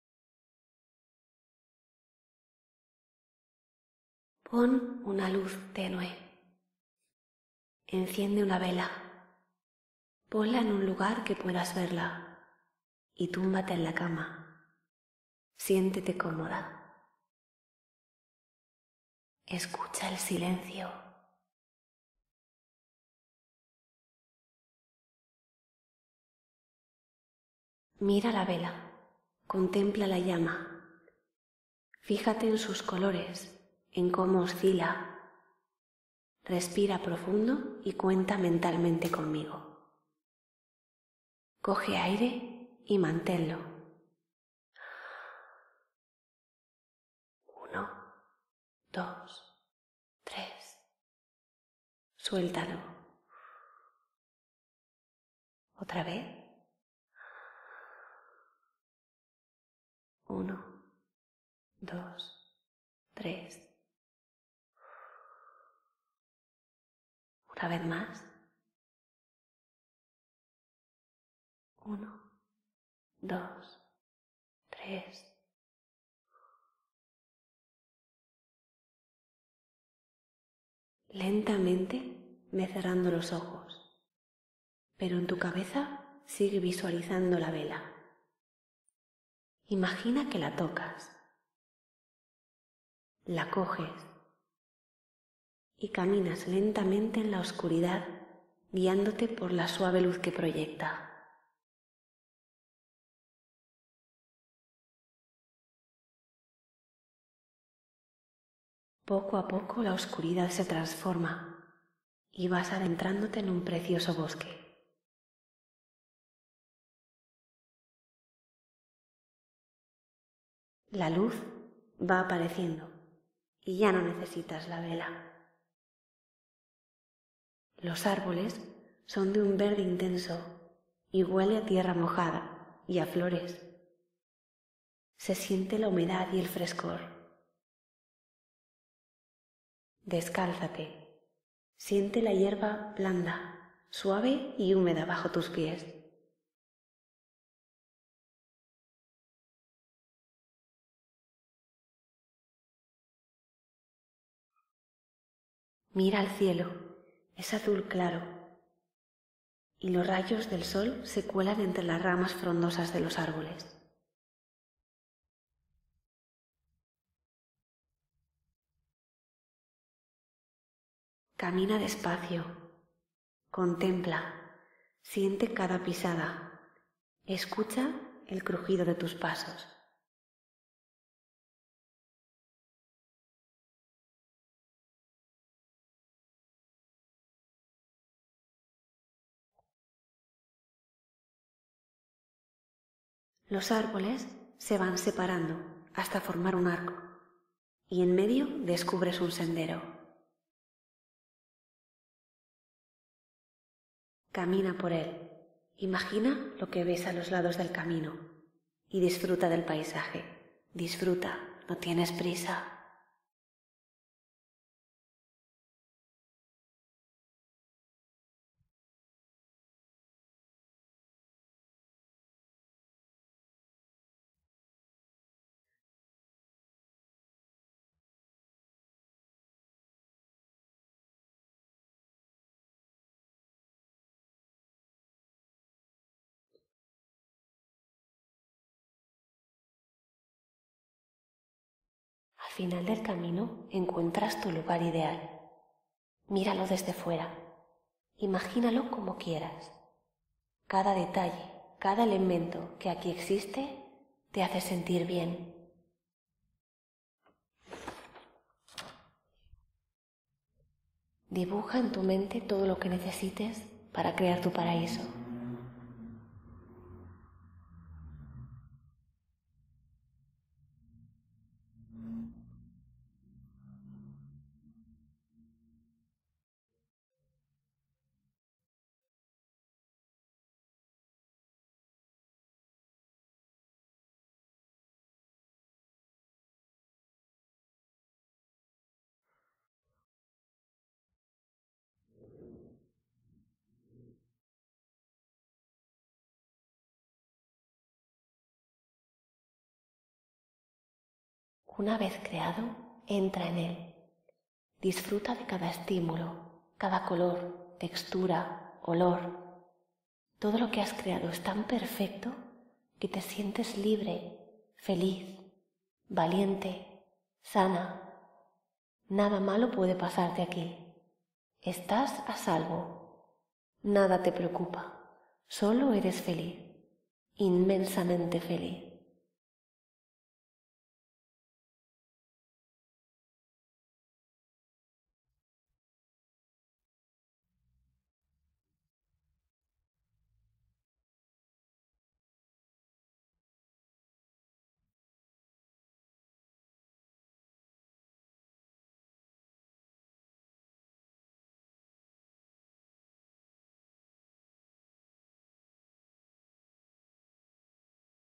Relajación guiada. Audio normal. ¡no susurros!
Déjate llevar, a través de mi voz, a un lugar relajante.
El momento de volver lo decides tú.